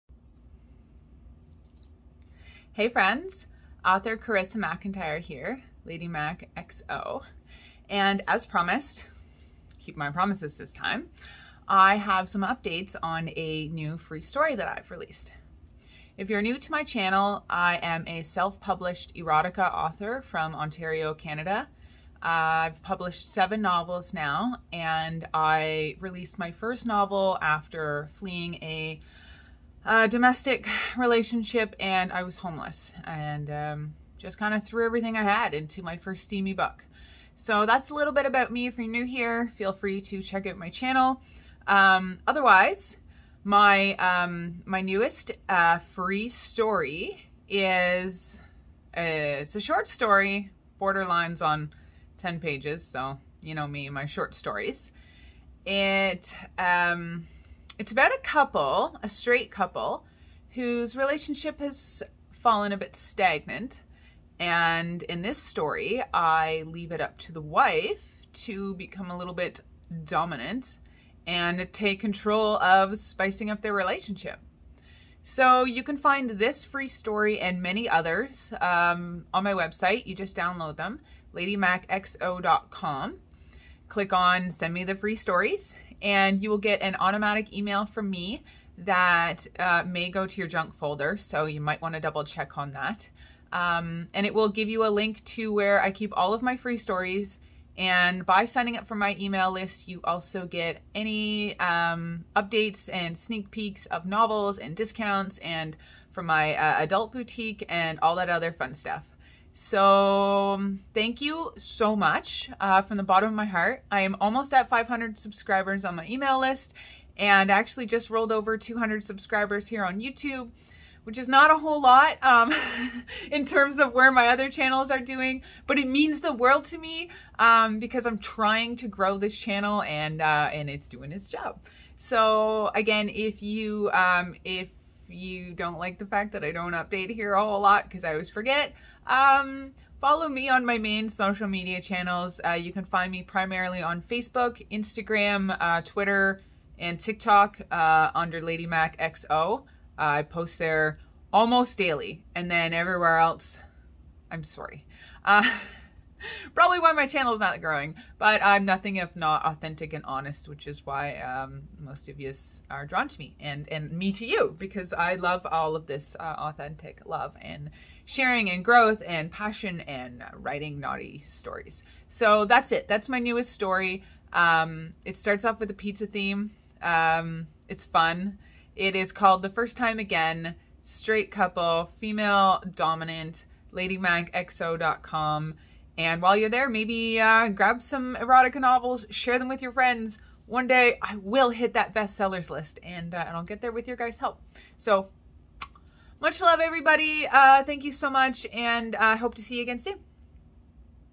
Do you like pizza? new free erotica short story! steamy adult bondage couples by Lady Mack xo
This is my newest free short story... it's about a couple whose marriage has fallen stale, so it's up to the wife to spice things up 🔥
does include bondage and oral

by Author Carissa McIntyre